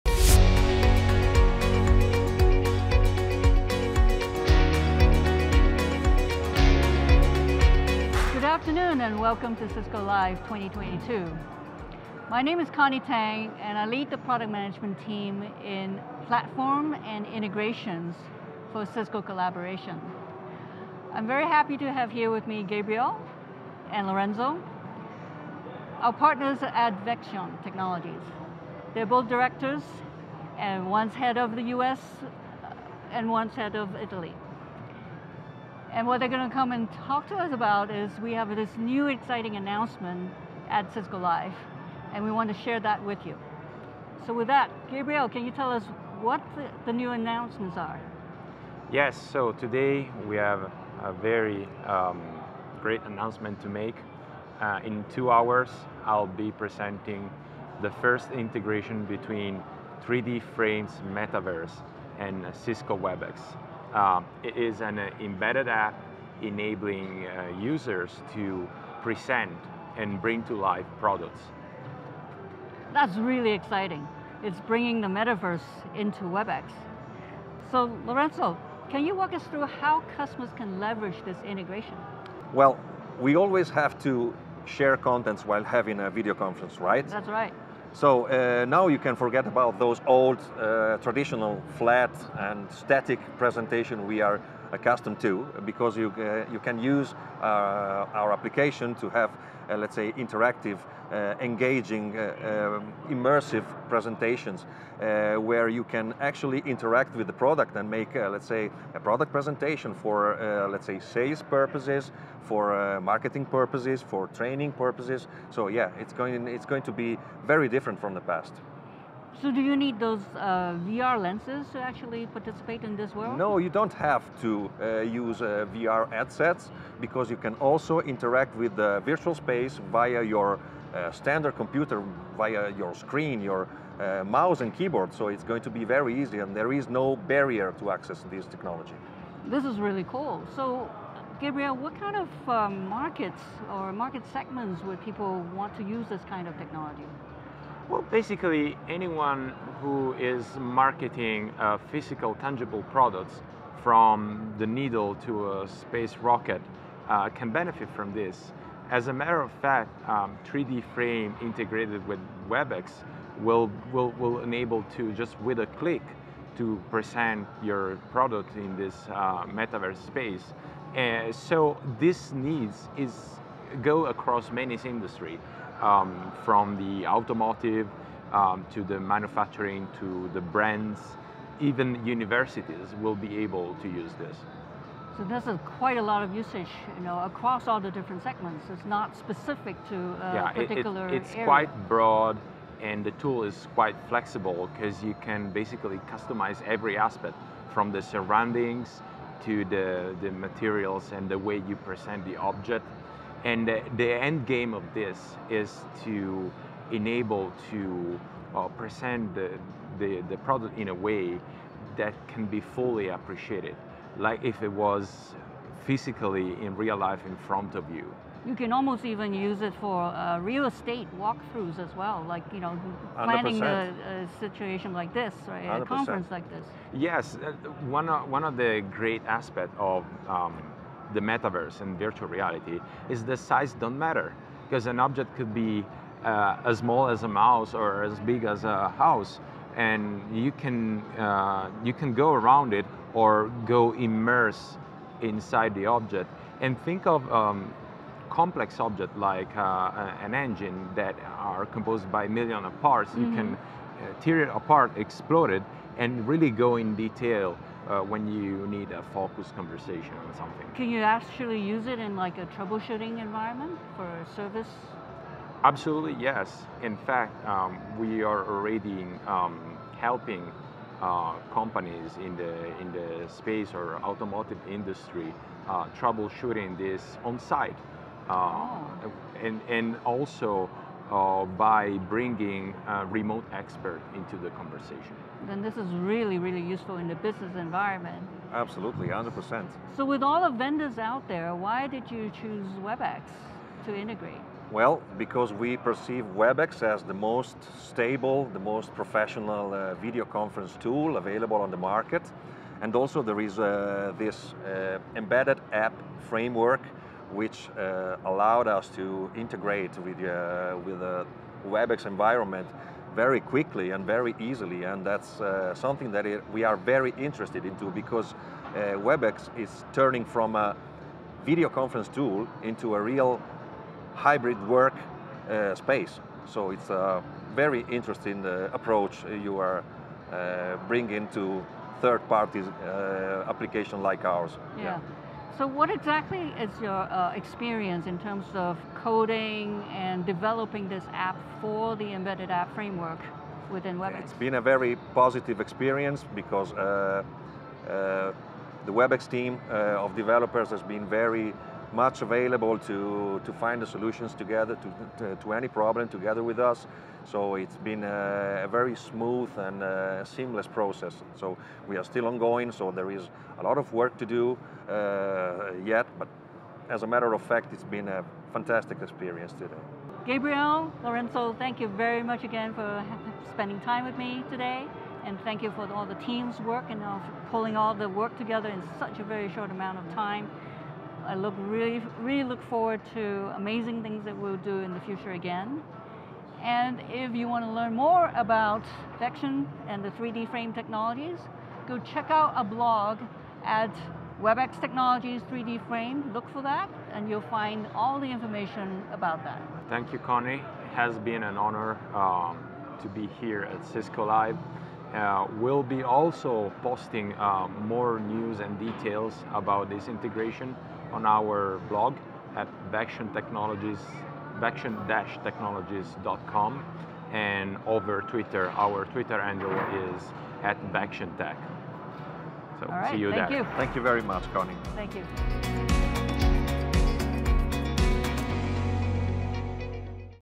Vection Technologies shares the 3D Frames metaverse integration with Cisco Webex as a partner.
Connie Tang interviews Gabriele Sorrento and Lorenzo Biagi regarding the 3D Frames metaverse integration with Cisco Webex announcement at Cisco Live and how developing on the platform was seamless